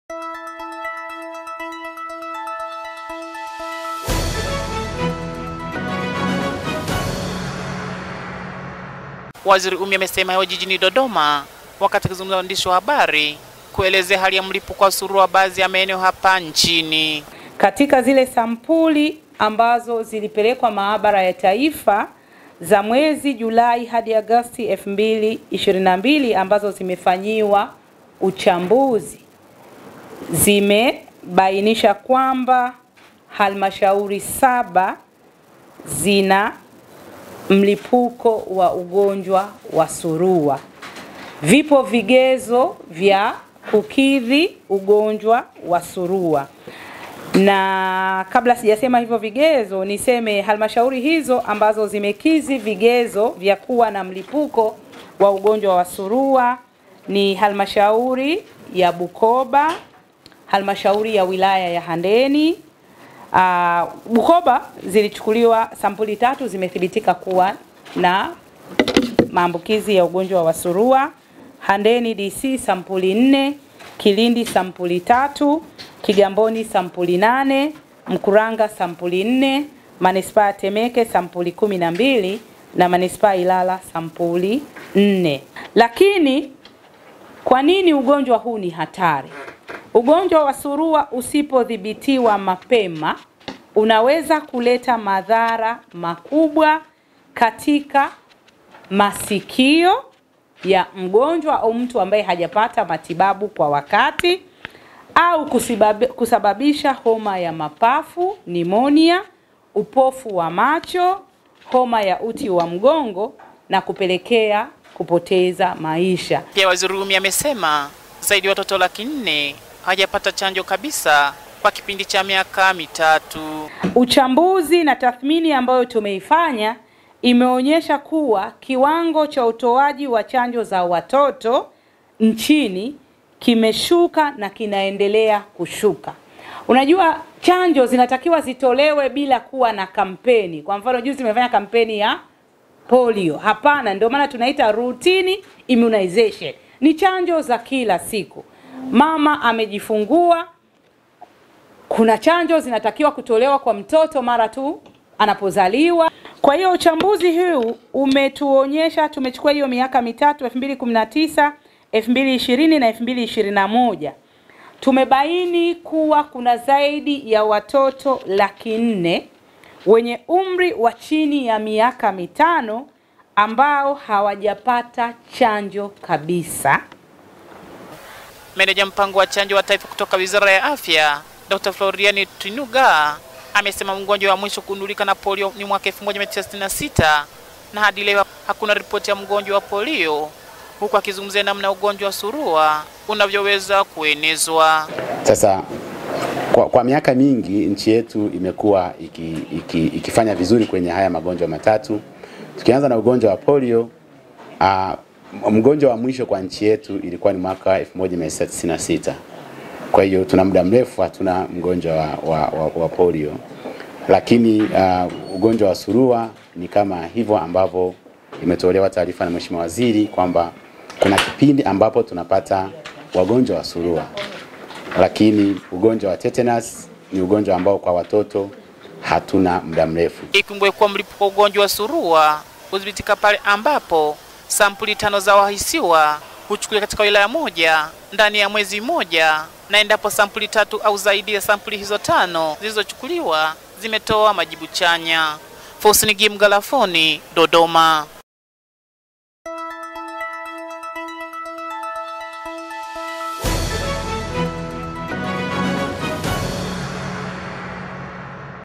WAZIRI UMMY ATAJA HALMASHAURI 7 ZENYE MLIPUKO WA SURUA
Waziri wa Afya,Ummy Mwalimu amesema pametokea ongezeko  la ugonjwa wa surua katika halmashauri 7.
Amesema kuna wagonjwa 38  walithibitika katika halmashauri za  Bukoba sampuli 3,Handeni sampuli 4,Kilindi sampuli 3,Mkuranga sampuli 4,Ilala sampuli 4,Temeke sampuli 12 na Kigamboni Sampuli 8.
Aidha,Waziri Ummya amesema idadi  ya wagonjwa wote  waliothibitika kuwa na maambukizi ya surua imefikia  54 kwa kipindi cha Julai hadi Agosti  huku hakuna taarifa ya kifo iliyoripotiwa kutokana na ugonjwa huo.